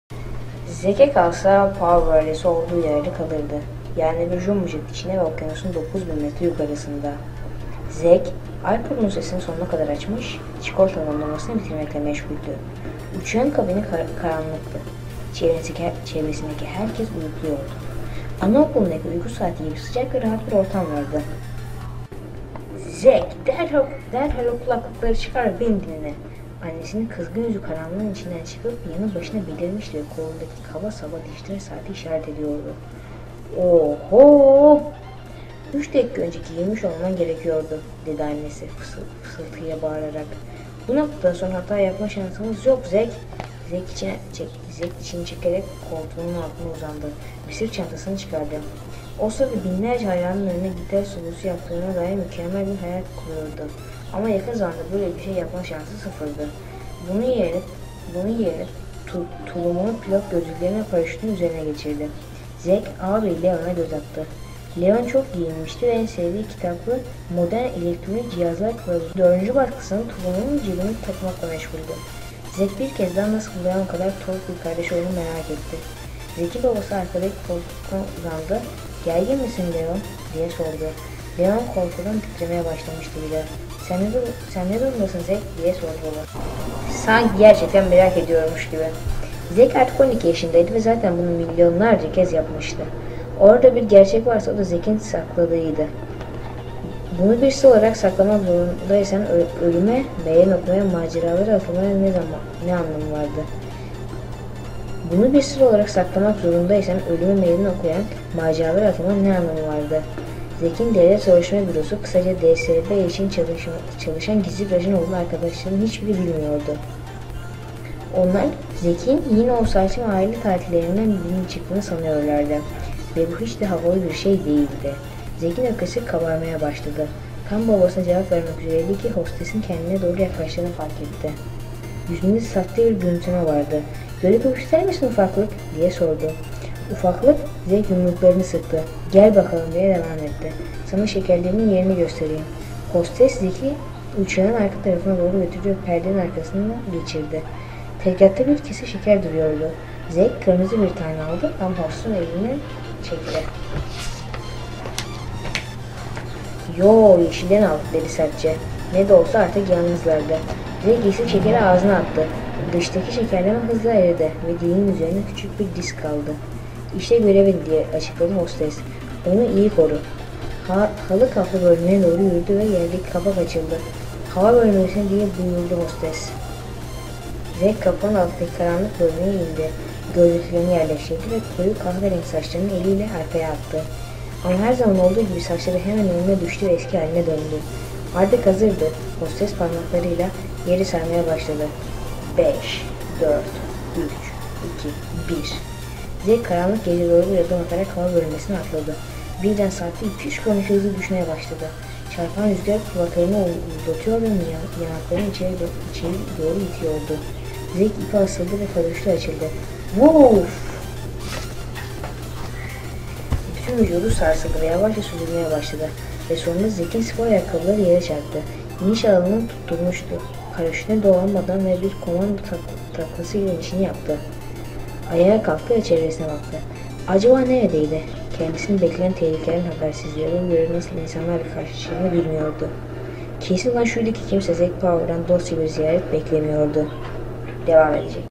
Zac Power 1 - Zehir Adası ( Sesli Kitap ) Bölüm 1
Yeni Nesil Süper Ajan Zac Power'la Tanışın

Dünyayı Kurtarmak Ve Ödevlerini Bitirmek İçin 24 Saati Var!

Zac Power, dünyanın en tehlikeli yerlerinden birine gittiğinde bu kez kardeşinin hayatını kurtarmak için zamana karşı yarışacağını bilmiyordu. Çok tehlikeli bir formül geliştiren

Dr. Drastic, Zac'in ağabeyini de esir almıştı. Zac'in görevi gizli formülü bulmak, Dr. Drastic'i etkisiz hale getirdikten sonra

ağabeyini kurtarmak ve tabii ki ödevlerini tamamlamak

için zamanında eve dönmekti...

Sayfa Sayısı: 92

Baskı Yılı: 2008

Dili: Türkçe
Yayınevi: Caretta Çocuk

Sayfa Sayısı : 92

İlk Baskı Yılı : 2008

Dil : Türkçe